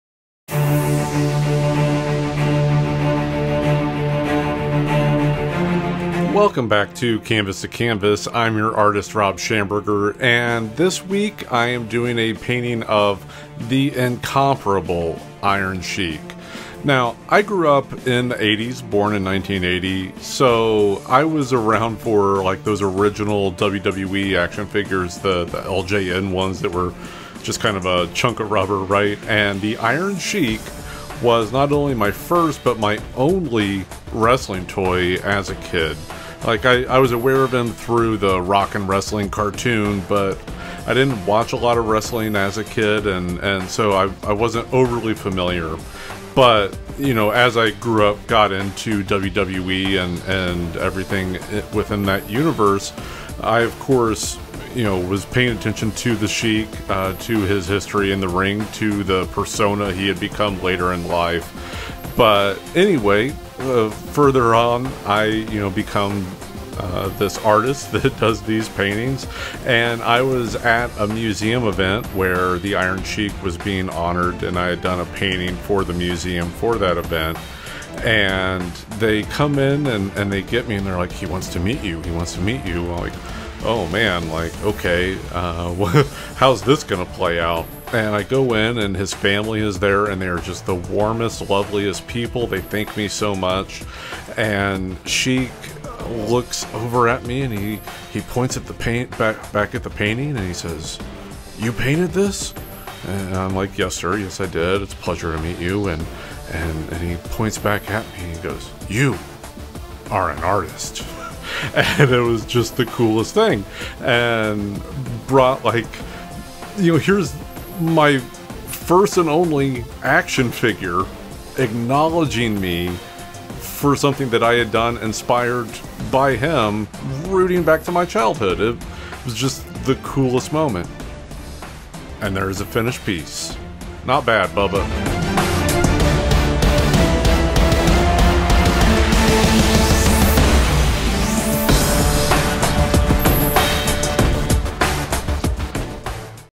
The Legacy of The Iron Sheik: WWE Canvas 2 Canvas
Artist Rob Schamberger paints a moving portrait of the late WWE Hall of Famer, The Iron Sheik, while recalling some of his storied career in wrestling.